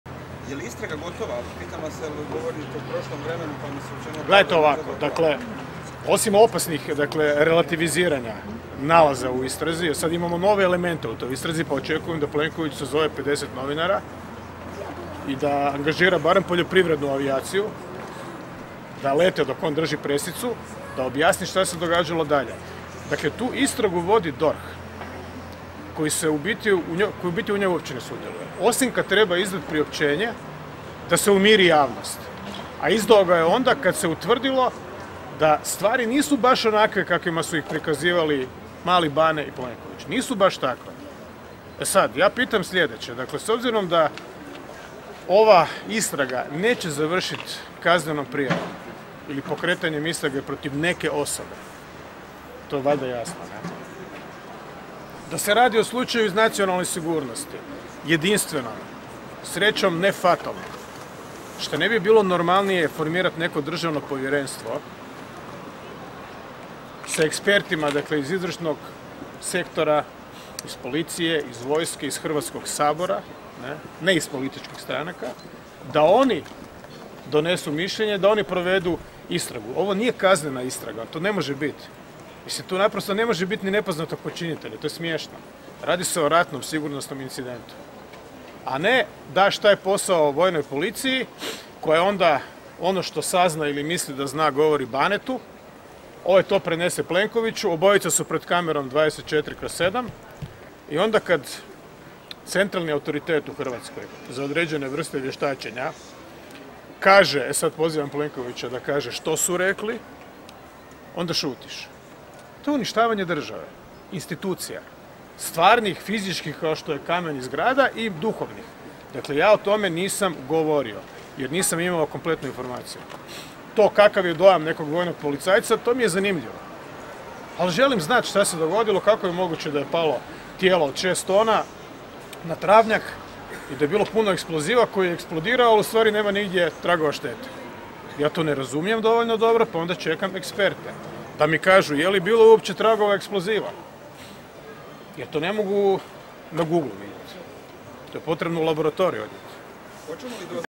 Milanović: Očekujem od Plenkovića da angažira poljoprivrednu avijaciju da lete dok on drži presicu
Nakon svečane sjednice povodom obilježavanja 20 godina osnivanja Sveučilista u Zadru,  predsjednik RH Zoran Milanović dao je izjavu medjima o istrazi o padu drona u Zagrebu.